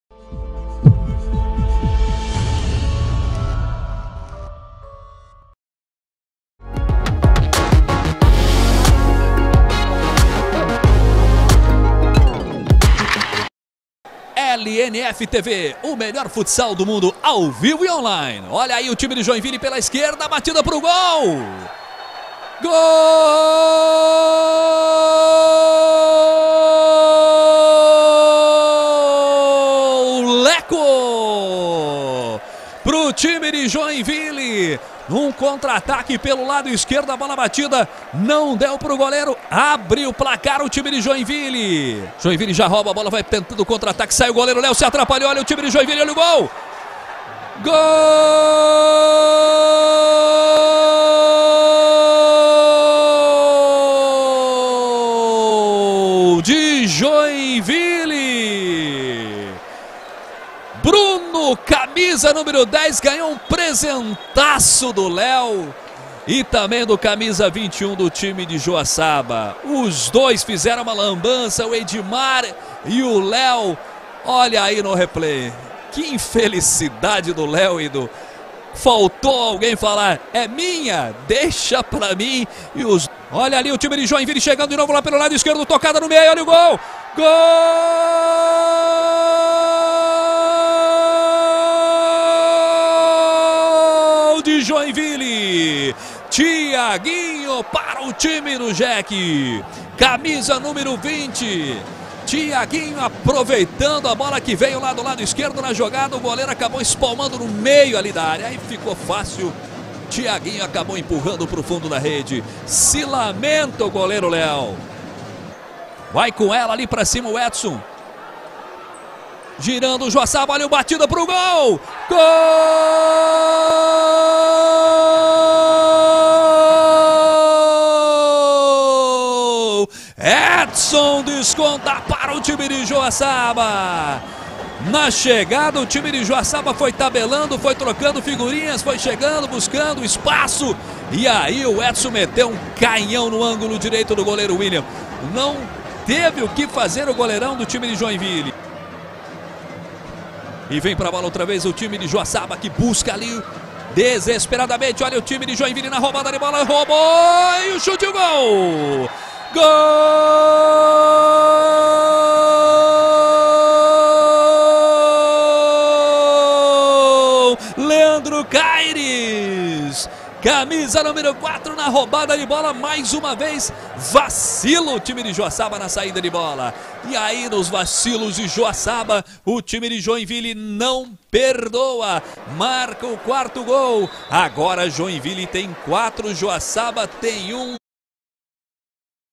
LNF2018 - Joaçaba 1 x 4 Joinville - Gols - 4ª Rodada
Gols da vitória do Joinville sobre o Joaçaba pela quarta rodada da LNF2018